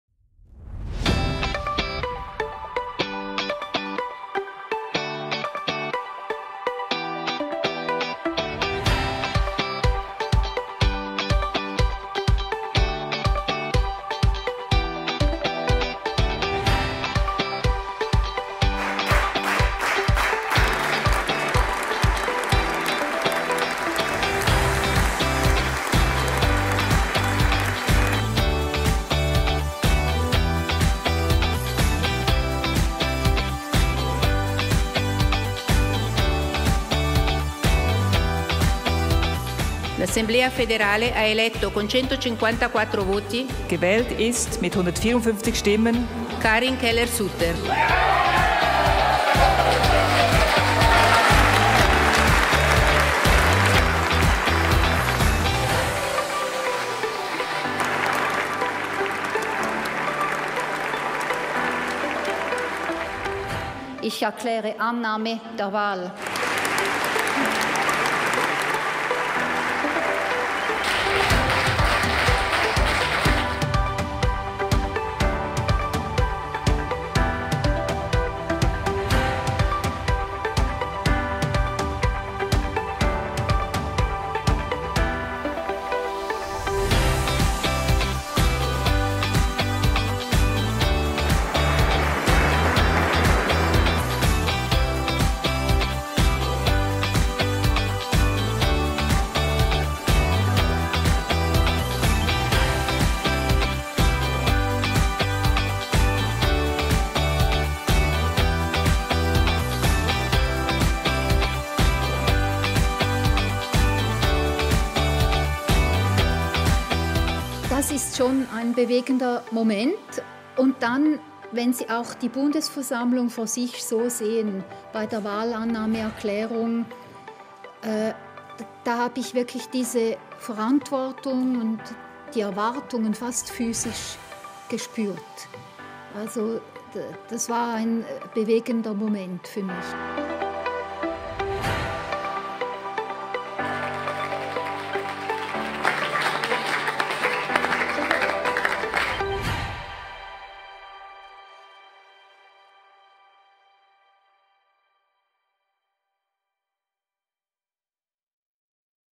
Elections au Conseil fédéral: Retour sur une journée historique
Près de deux mois et demi après l’annonce de départ de Johann Schneider-Ammann, l’Assemblée fédérale a élu aujourd’hui Karin Keller-Sutter et Viola Amherd, deux femmes, au Conseil fédéral. Nous sommes ravis et remercions le conseiller aux États Hans Wicki pour son excellente candidature.  Retour en images sur une journée historique et riche en émotions.